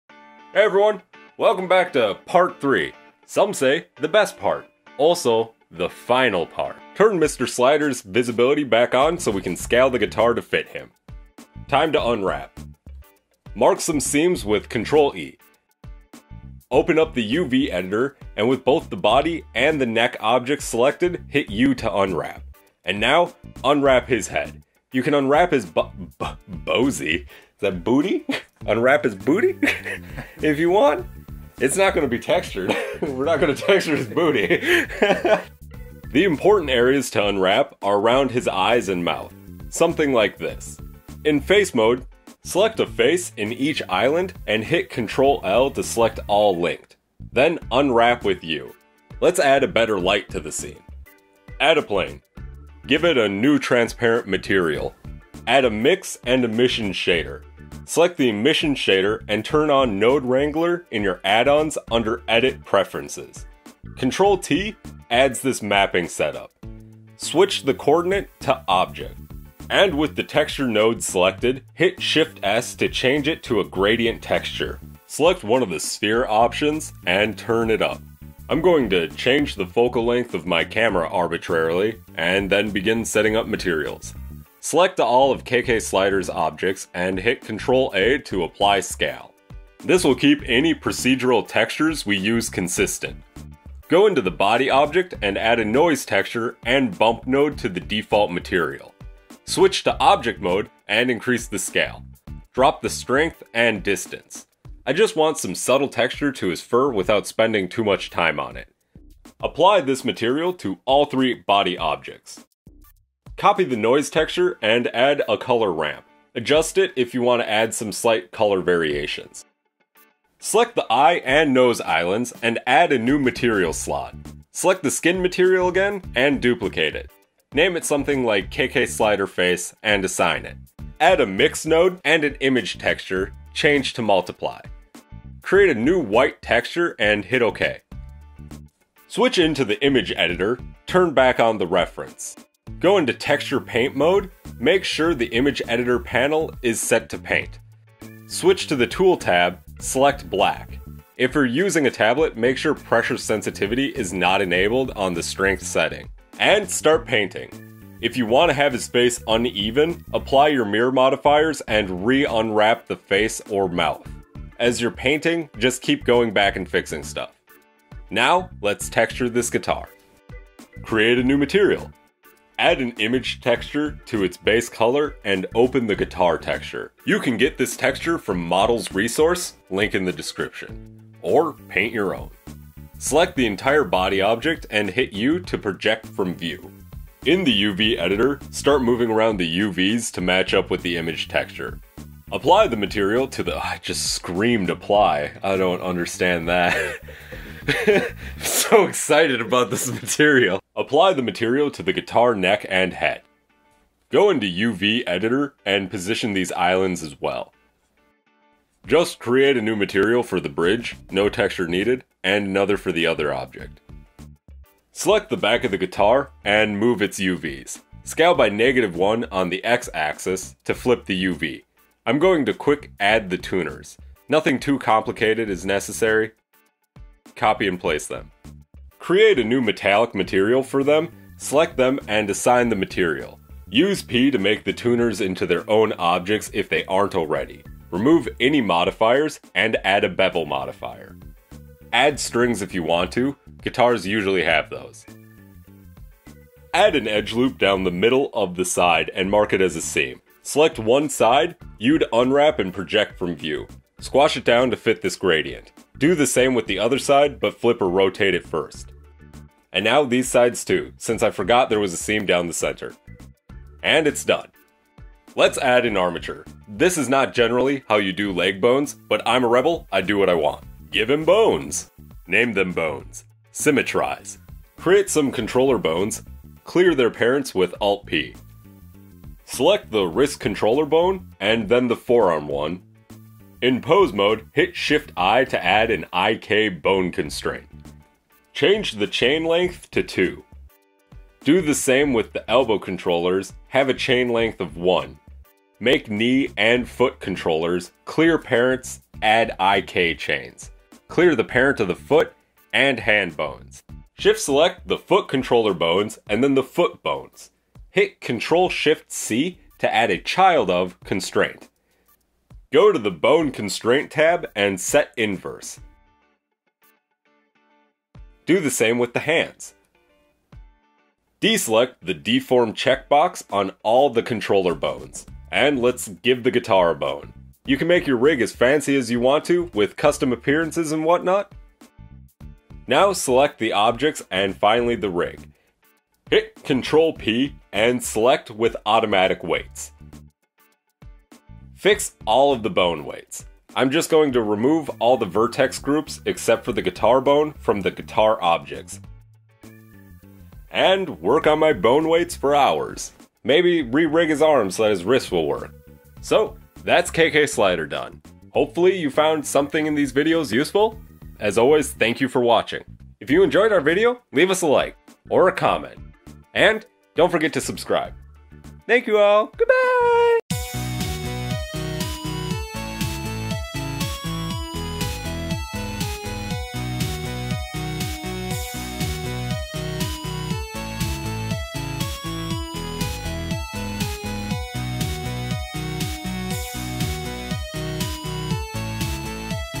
How to 3D Model K.K. Slider in Blender [Part 3:Final]
This is part 3 of our tutorial on how to create KK Slider from Animal Crossing New Horizons. In part 3 we texture and rig our model! I hope you guys find it useful!